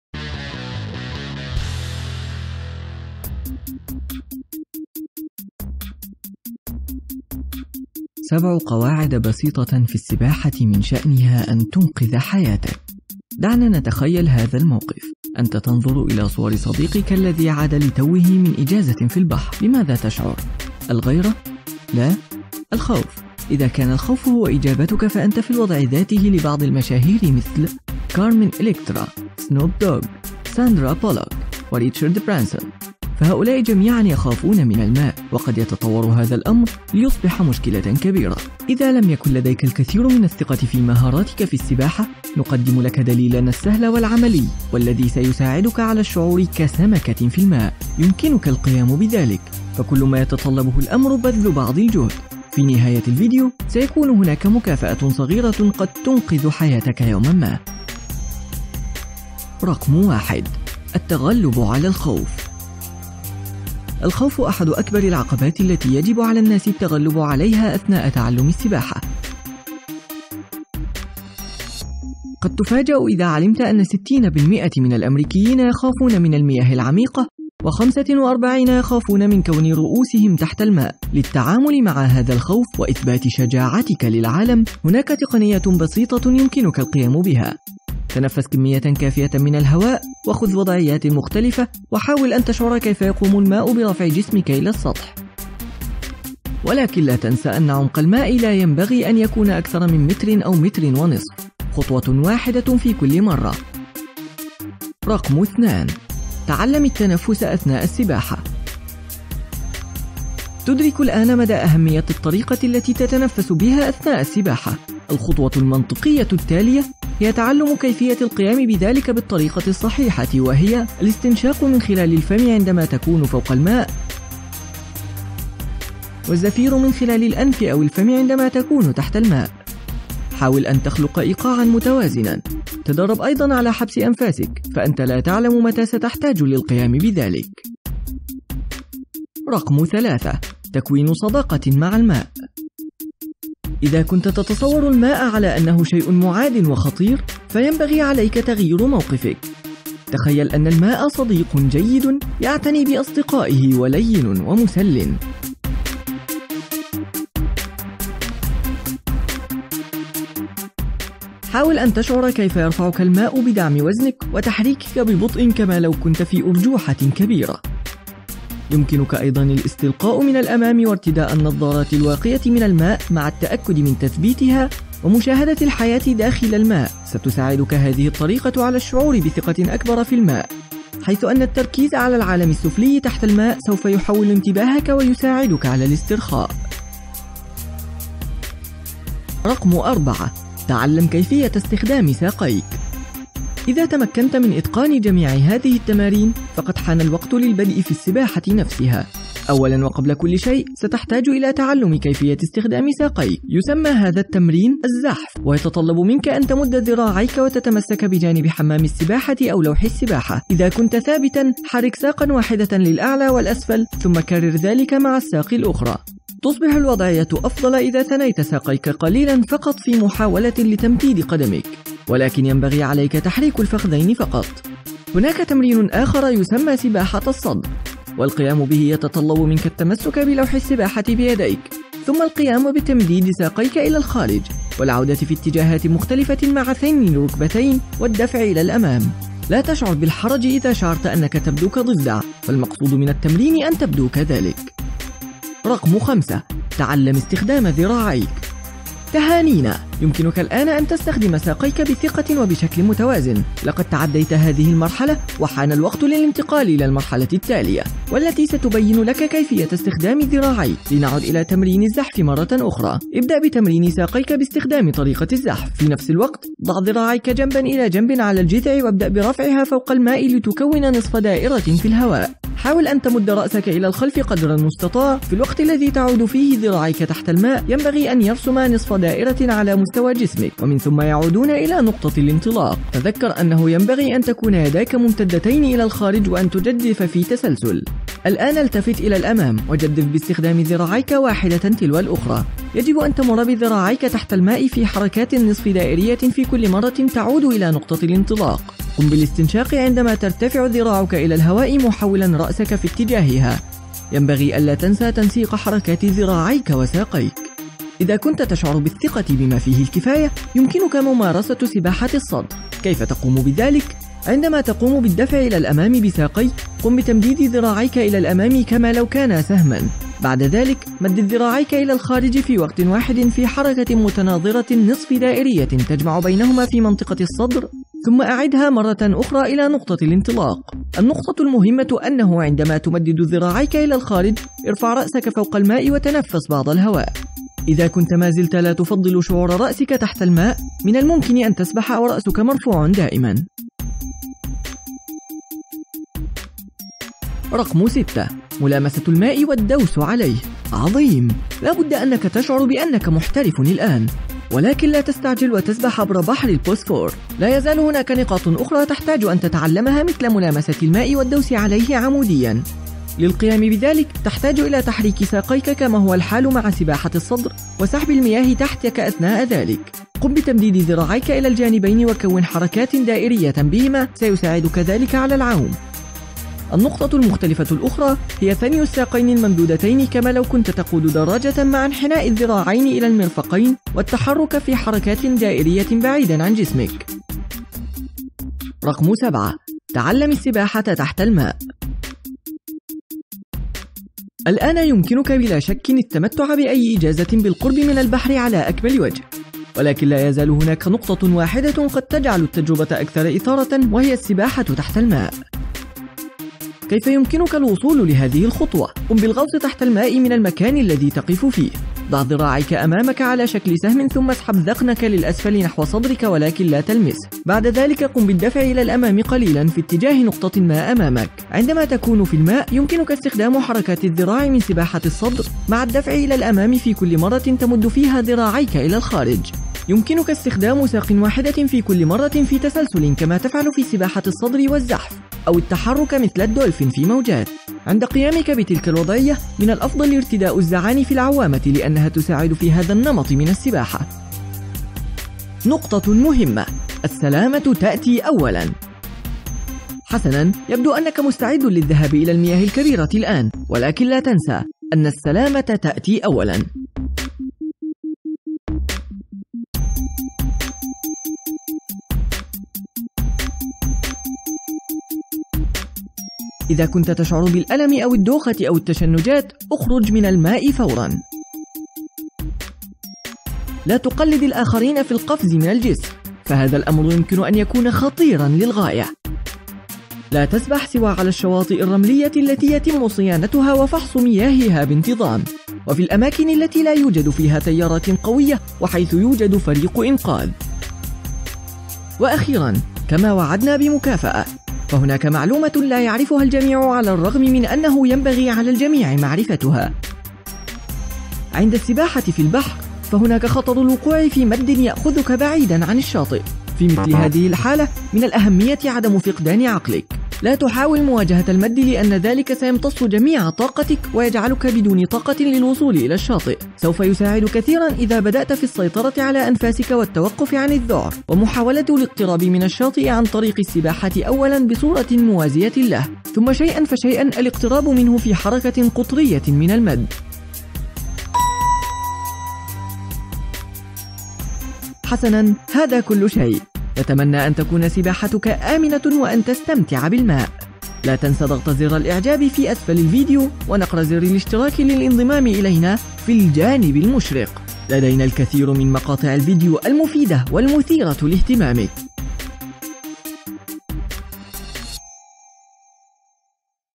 سبع قواعد في السباحة من شأنها أن تنقذ حياتك
إذا كنت لا تمتلك الكثير من الثقة في مهاراتك في السباحة، نقدم لك دليلنا السهل والعملي والذي سيساعدك على الشعور كسمكة في الماء.